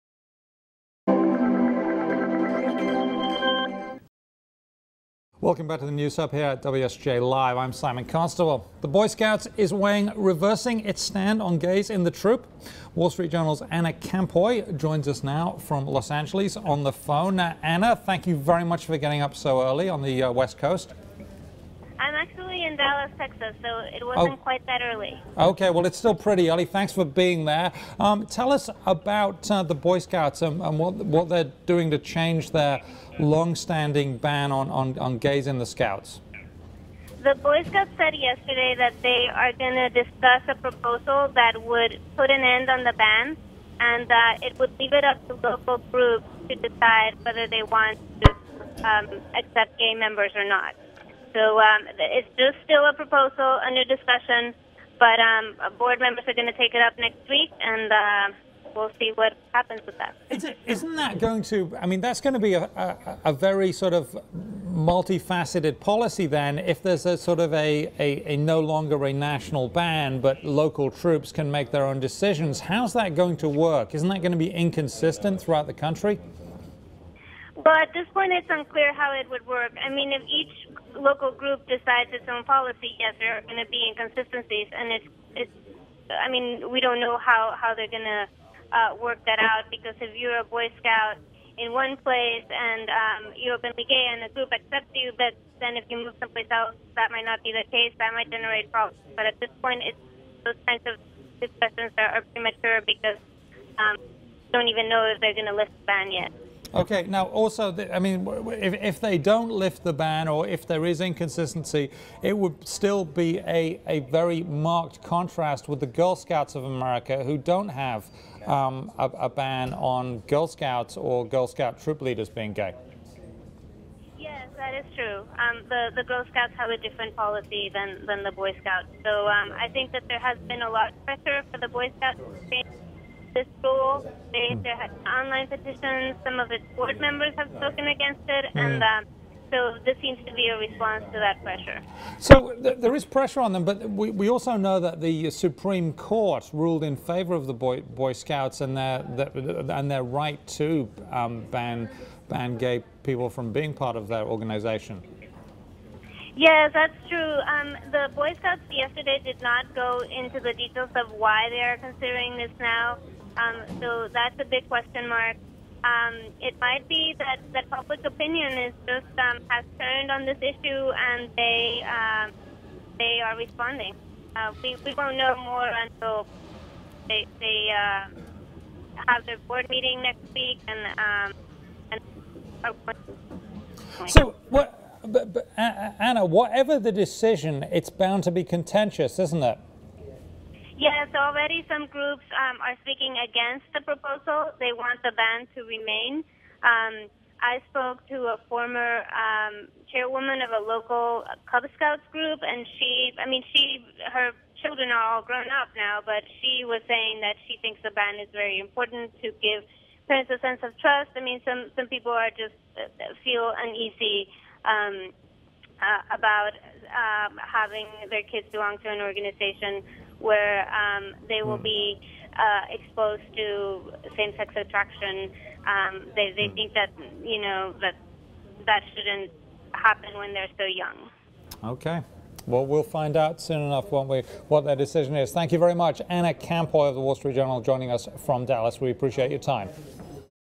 Boy Scouts Rethink Gay Ban
The Boy Scouts of America is weighing an about-face to its long-standing ban on gays among its membership and troop leaders. WSJ's Ana Campoy joins the News Hub to discuss the latest details. Photo: Getty Images.